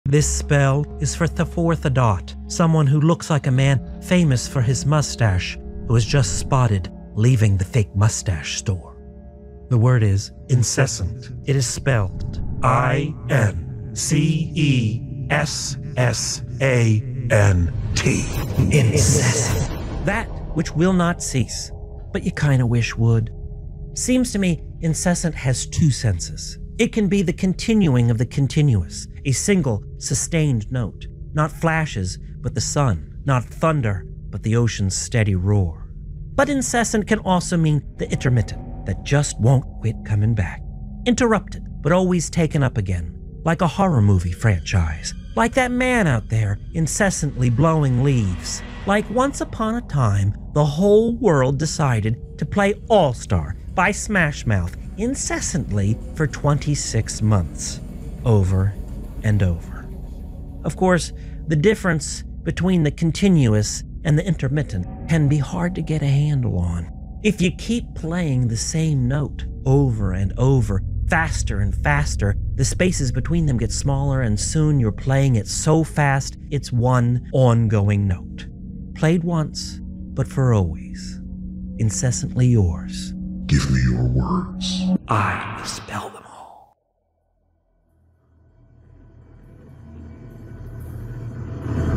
Wilder Spells "Incessant"
This spell is for @the4thdot Give me your words. I will spell them all. No limit. In the comments preferred!

Speaking of incessant, I have had a cold for about a week now. Before that I was well for about two weeks. And then before that, an unbroken chain of colds going back to Covid in January. Having kids in school is no fun for the immunocompromised. If you ever bargain for life everlasting, be sure to demand immunity to illness.

Also, this spell contains a true story about @scribejr. I hope its powers free you of the Incessant Blower.